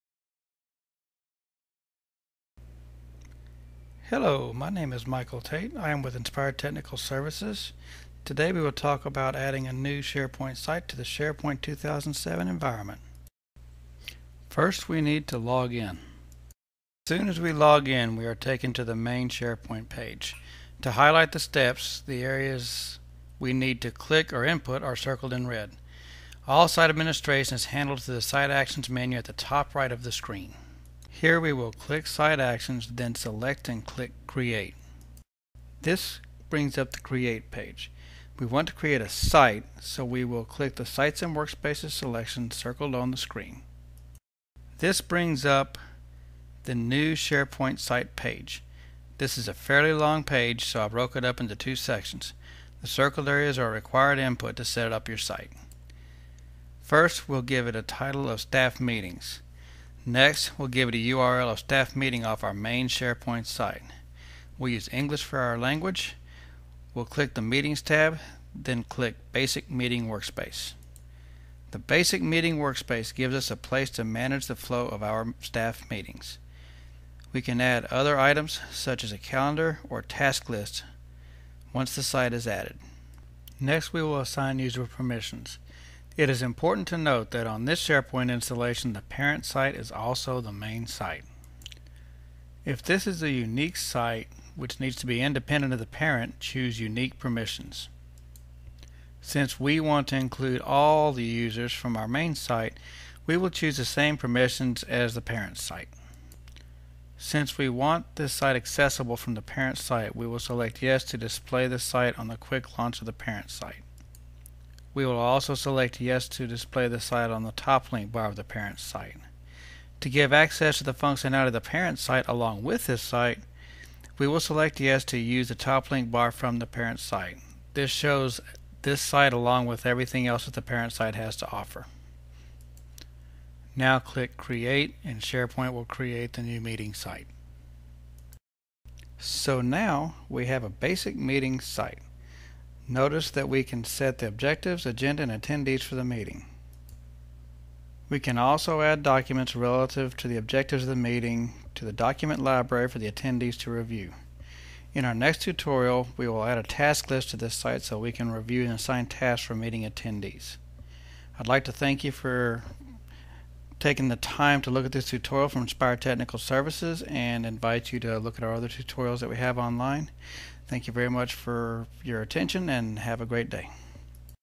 SharePoint Staff Meeting Site
Creation of a meeting site in SharePoint